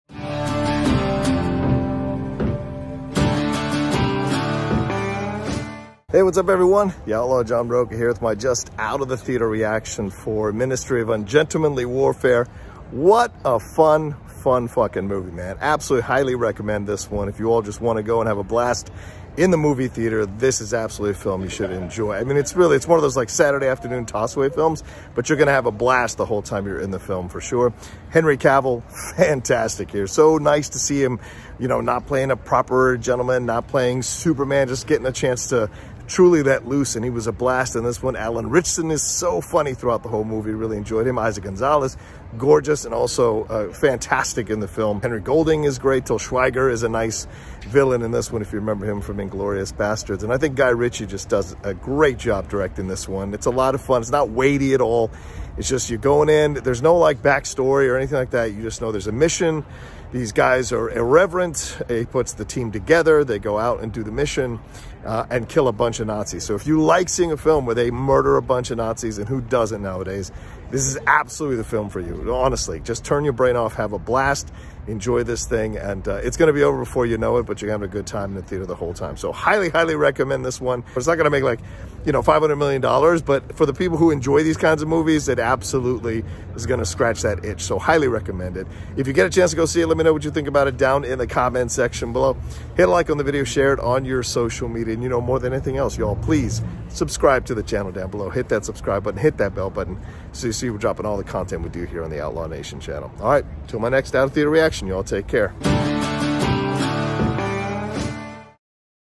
THE MINISTRY OF UNGENTLEMANLY WARFARE Out of The Theater REACTION!! | Guy Ritchie | Lionsgate
The Outlaw John Rocha brings you his OUT OF THE THEATER Reaction for THE MINISTRY OF UNGENTLEMANLY WARFARE from director GUY RITCHIE and LIONSGATE. The British military recruits a small group of highly skilled soldiers to strike against German forces behind enemy lines during World War II. The film stars HENRY CAVILL, ALAN RITCHSON, EIZA GONZALEZ, CARY ELWES, ALEX PETTYFER and HENRY GOLDING. It's out in theaters this weekend on April 19th!